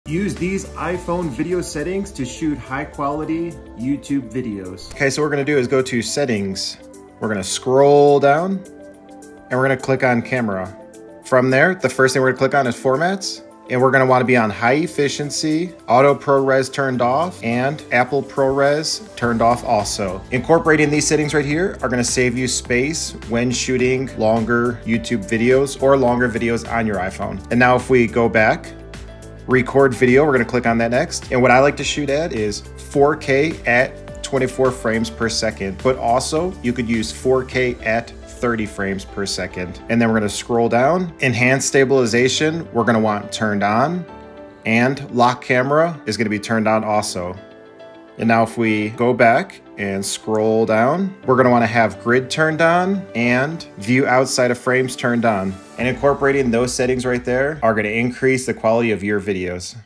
USE THESE IPHONE SETTINGS TO SHOOT HIGHER-QUALITY VIDEOS!!
In this short, I show you how to shoot higher-quality videos with your iPhone by enabling these settings.